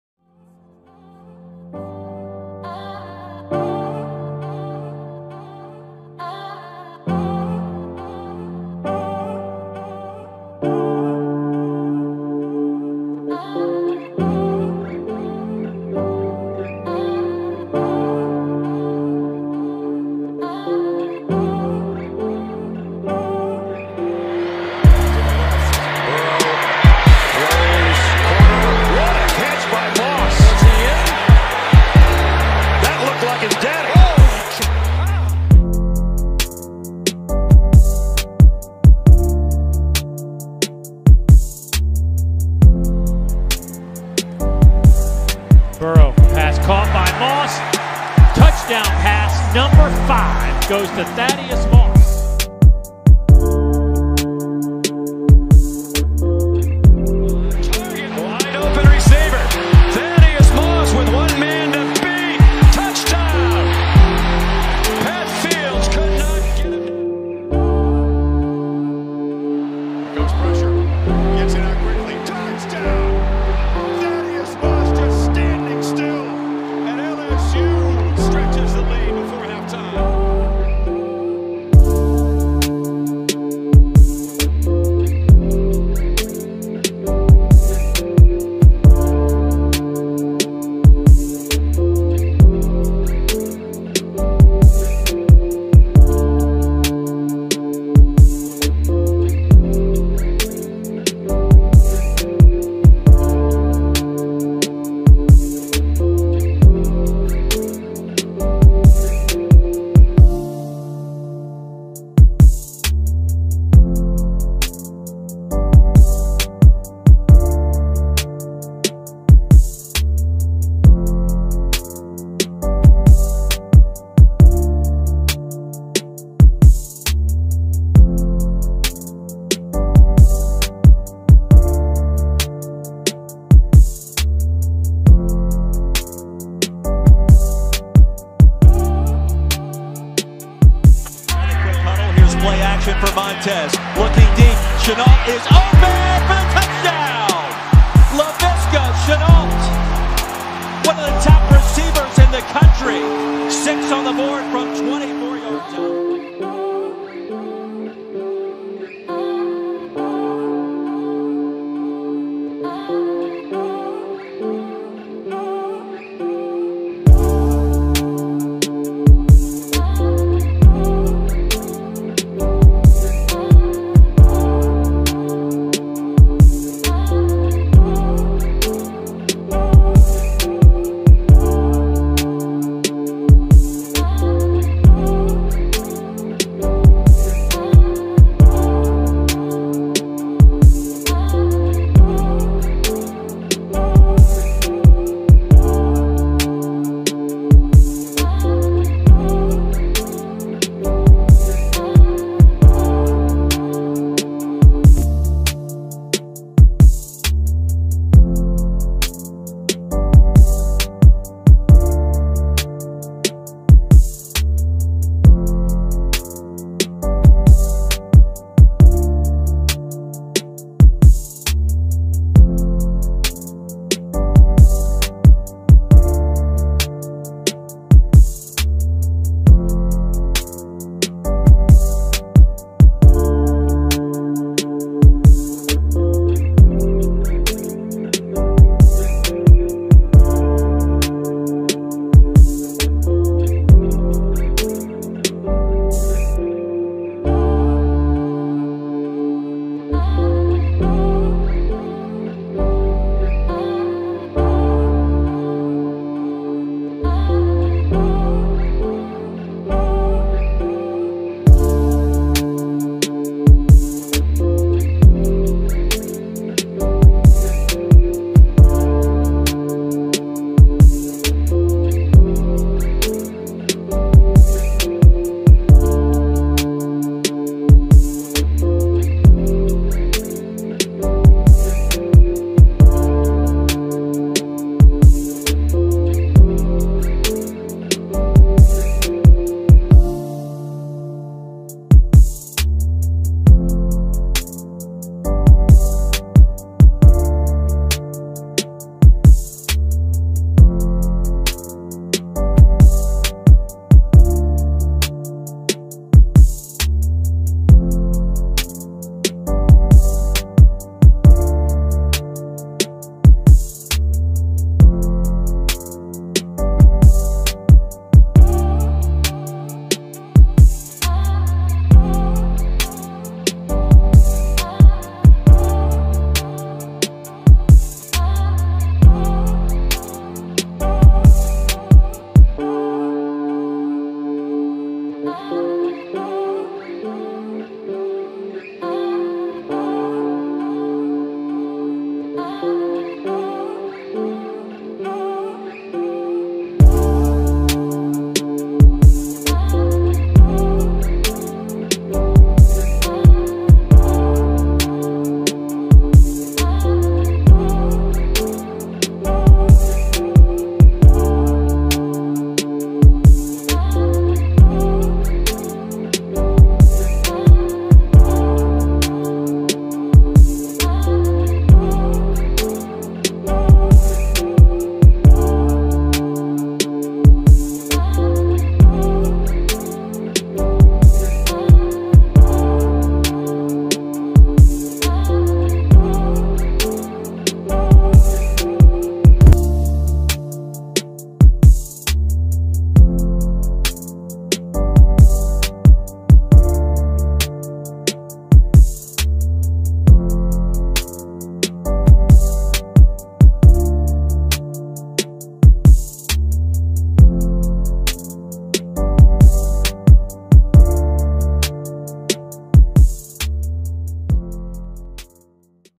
II "DIAMONDS IN THE ROUGH" II Washington Redskins UDFA class breakdown w/ highlights
A complete breakdown of the 2020 UDFA haul for the Washington Redskins with highlights